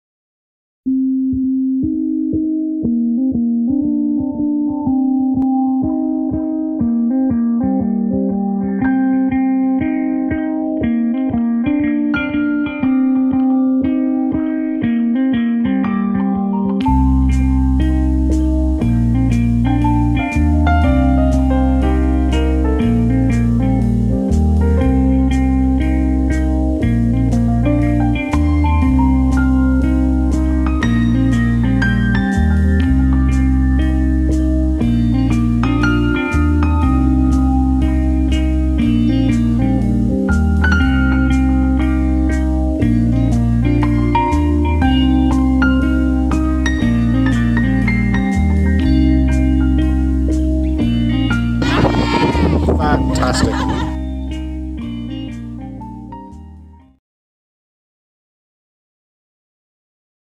DEEP Chapter 1: Exploring ATMEC!
2020 and early 2021 saw the remarkable kids from DEEP, from the American School of Bangkok, Green Valley campus visit ATMEC at Koh Mun Nai and pursue their field-research education!

All photo and video is property of ATMEC unless clearly stated otherwise.